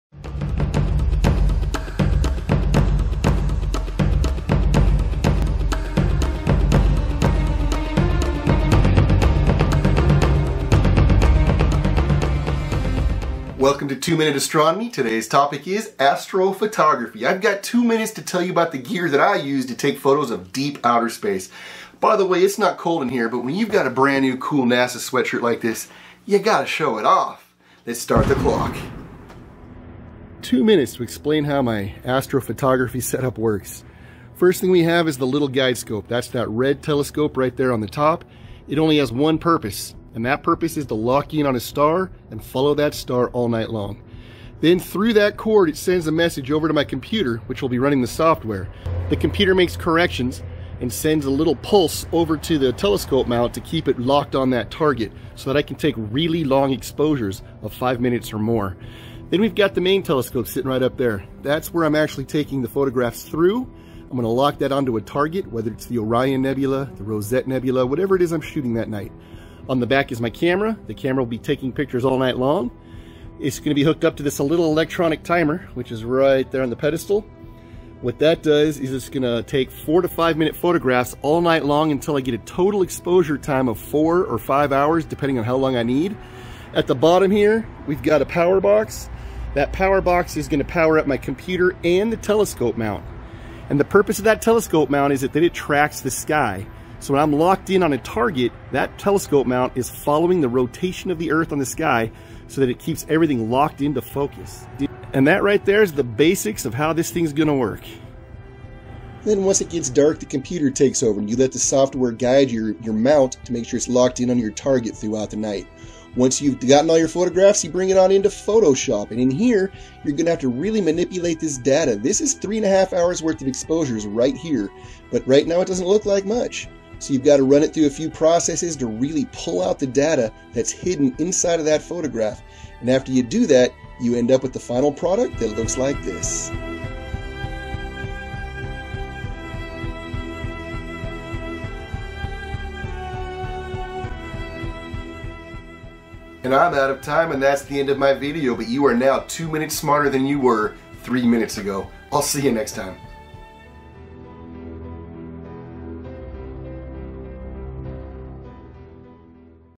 Two Minute Astronomy: Introduction to Astrophotography
The Two Minute Astronomy series is designed to give a quick look at some cool aspect of astronomy.

In this video, I demonstrate the basics of my astrophotography gear (Sky-Watcher EvoStar 72 ED, Sky-Watcher EQM-35 mount, SVbony guide scope ... etc.)